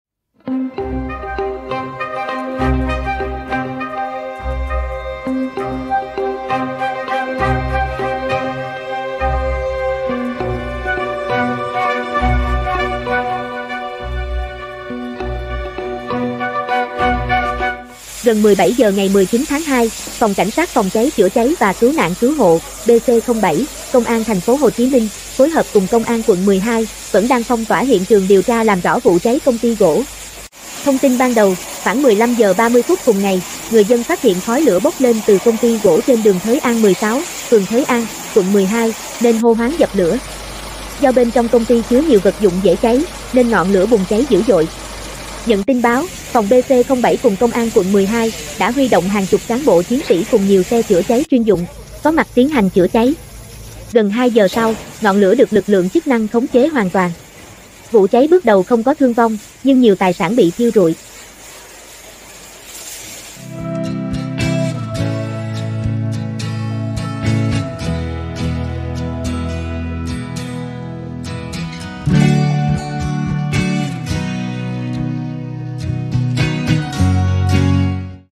Cháy lớn tại công ty gỗ ở quận 12, TP HCM
Thông tin ban đầu vào khoảng 15h30 ngày 19-2, công ty chuyên gỗ sấy nhập khẩu trên đường Thới An 16, phường Thới An, quận 12 (TP.HCM) bất ngờ bốc cháy dữ dội. Khói lửa bốc lên ngùn ngụt, bao trùm khu vực.

Người dân xung quanh tìm cách dập lửa tại chỗ nhưng bất thành. Ngọn lửa bùng phát mạnh hơn. "Ngọn lửa đỏ rực cuồn cuộn bốc lên khi đám cháy bén vào gỗ, mùi bốc ra khét lẹt", một người dân gần hiện trường chứng kiến cho hay.

Nhận tin báo, lực lượng Cảnh sát PCCC và cứu nạn cứu hộ (PC07) Công an TP.HCM và PCCC quận 12 nhanh chóng có mặt tại hiện trường triển khai công tác chữa cháy.